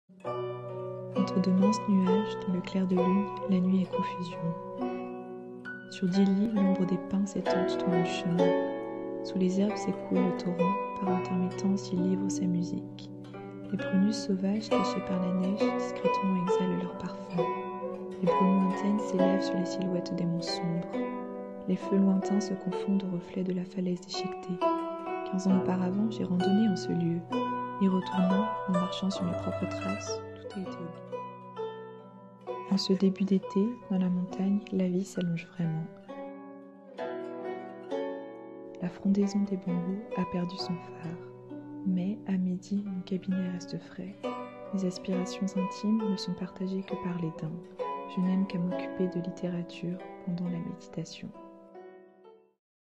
La peinture et la poésie chinoise | WenZhengming文徵明 & Tang Yin唐寅
Poésie et peinture chinoises 中国诗画

Wen Zhengming (chinois : 文徵明 ; pinyin : wén zhēngmíng), calligraphe et poète chinois de la dynastie Ming, né en 1470, et originaire de Suzhou, dans la province du Jiangsu, mort en 1559. Né Wen Bi (文壁), il prit pour nom personnel (zi/字) Zhengming, comme surnom Zhengzhong (徵仲), et comme nom de pinceau (hao/号) Héngshān jūshì (衡山居士).

Tang Yin（chinois : 唐寅 ; pinyin : táng yín), surnoms : Bohu(伯虎) et Ziwei (子畏), noms de pinceau : Liuru Jushi(六如居士) et Taohua Anzhu(桃花庵主), né en 1470, originaire de Suzhou, province de Jiangsu, mort en 1523. Chinois. Peintre de genre, figures, paysages, fleurs.